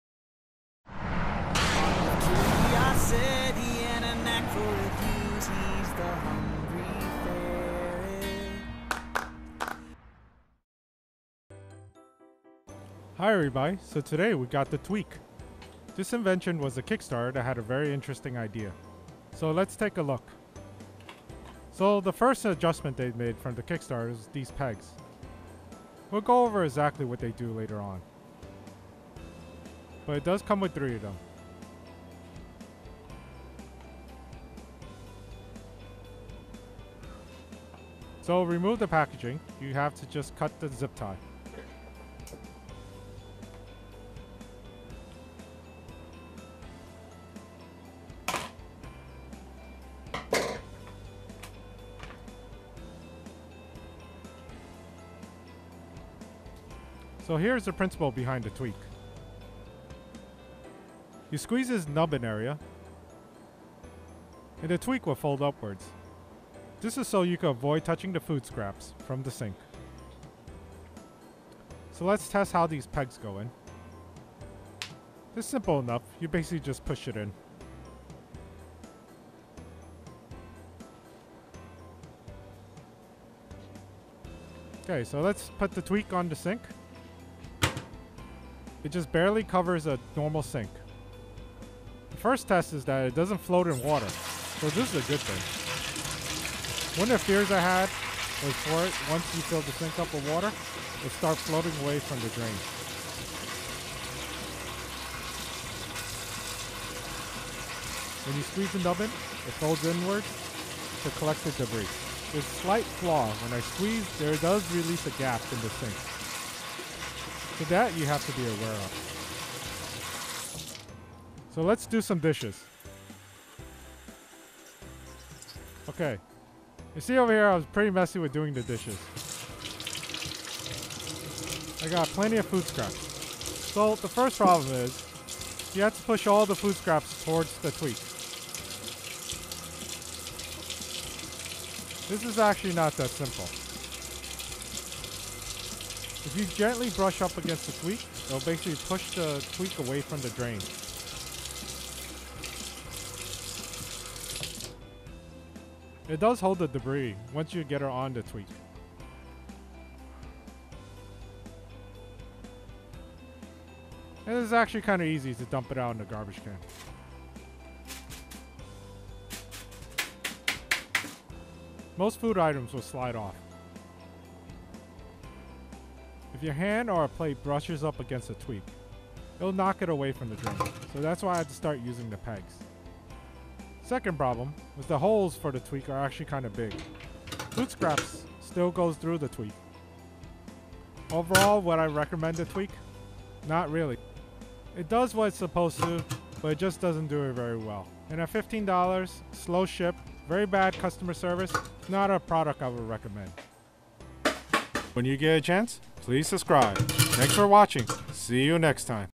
TWEAK the Sink waste catcher [Kickstarter]
I had high hopes for this product, it proved more effort than it saved in my case.  It is well made but, it just didn't work as well as I hope for it to.